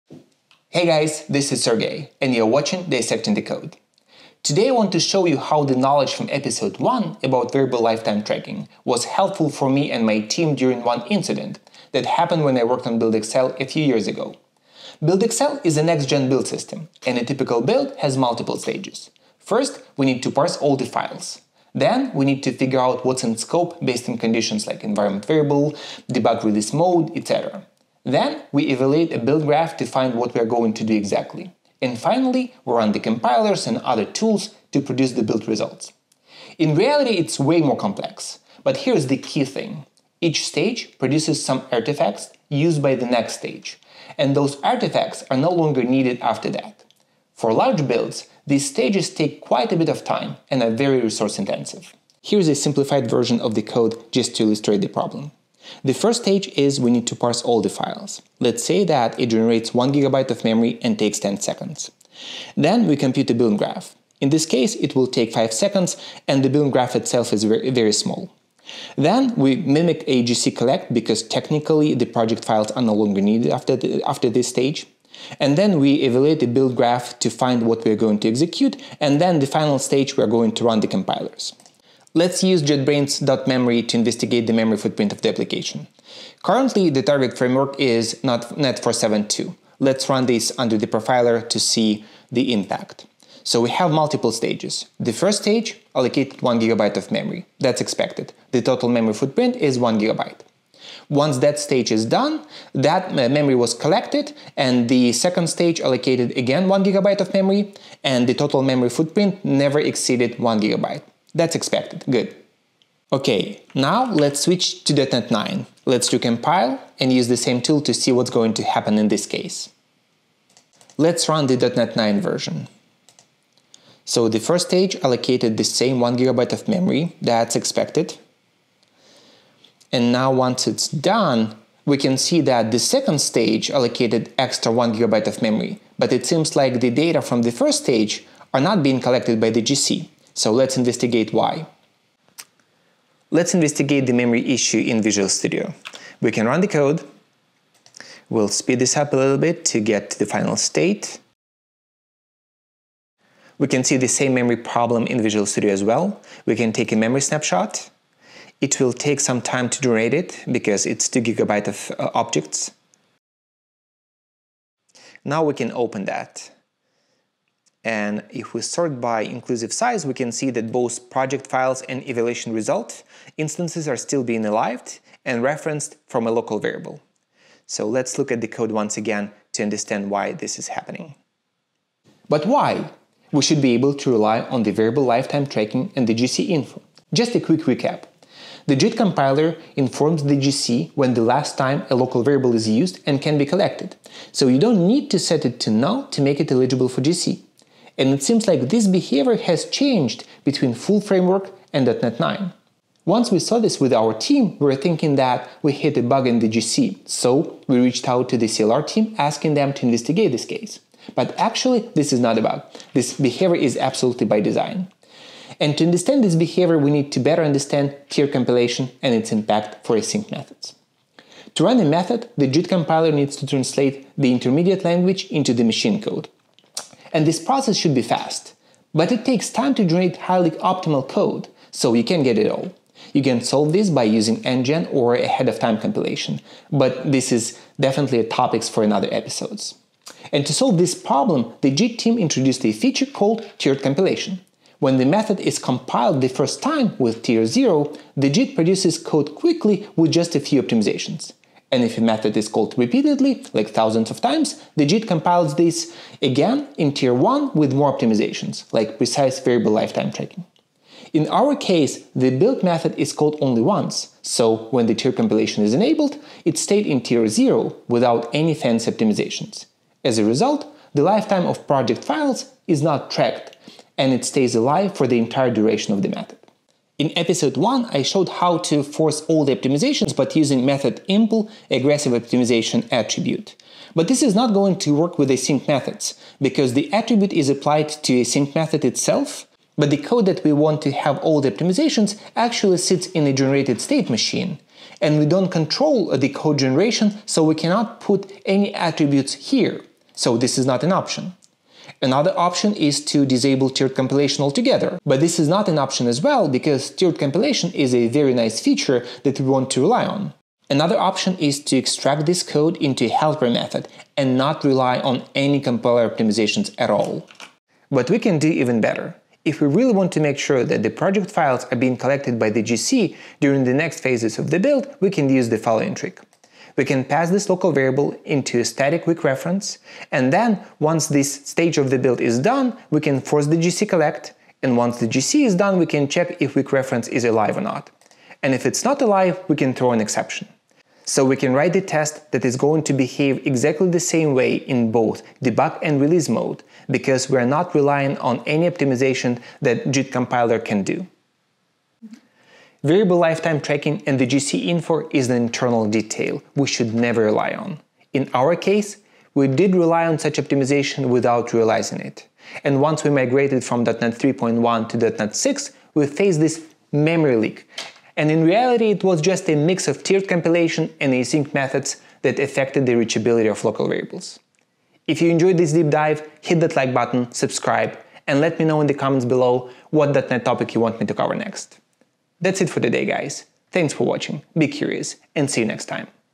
Can Tiered Compilation Cause Memory Leaks in .NET?
In this episode of Dissecting the Code, I share how my team uncovered a surprising memory leak after migrating from .NET Core 3.1 to .NET 6. We revisit how tiered compilation affects local variable lifetime in async methods - and why it caused our GC behavior to change unexpectedly. If you're upgrading .NET or dealing with memory bloat, this deep dive is for you.

 00:00 – Intro: BuildXL overview
 00:53 – Simplified version of the build process
 01:22 – Investigating memory usage for net472 app with JetBrains DotMemory
 01:55 – Investigating memory usage for net9 app
 03:05  – Tiered Compilation explained
 04:44 – How to fix the "memory leak"
 05:20 – Preventing the issue from happening
 05:55 –  Final Thoughts

👇 Drop your topic suggestions in the comments and let’s keep dissecting together.